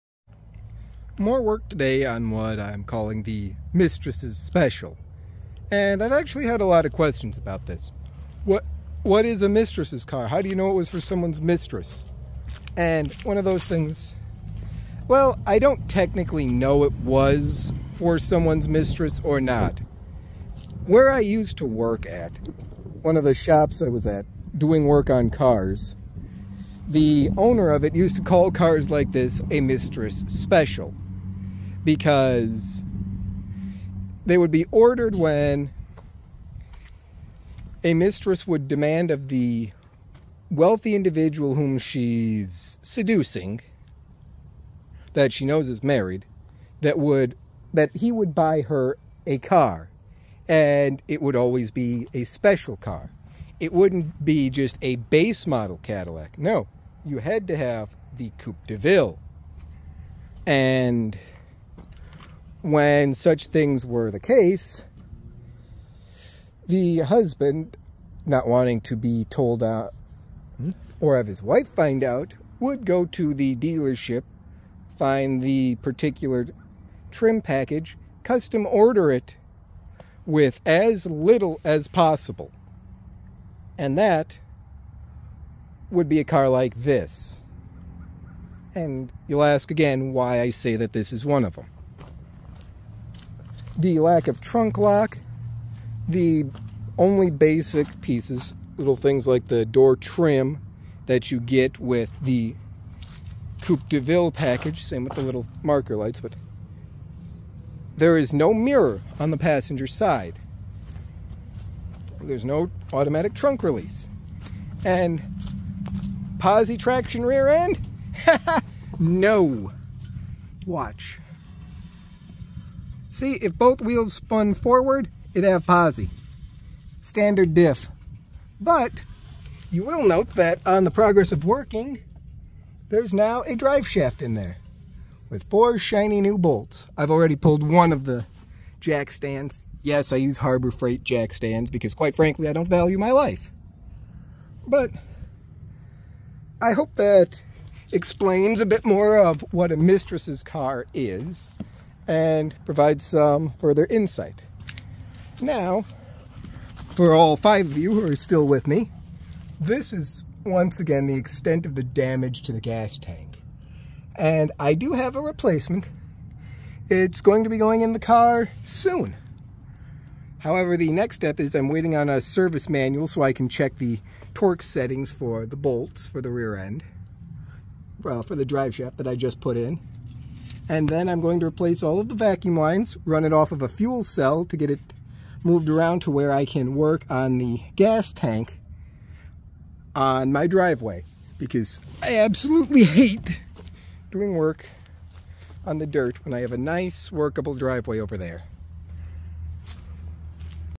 1971 Cadillac - What is a "Mistress's Car" and Posi Test
automobile #1971 cadillac mechanic posi cadillacdeville mycarruns classiccadillac Answering the question I've been getting lately, plus, testing the car for positraction while the rear is in the air.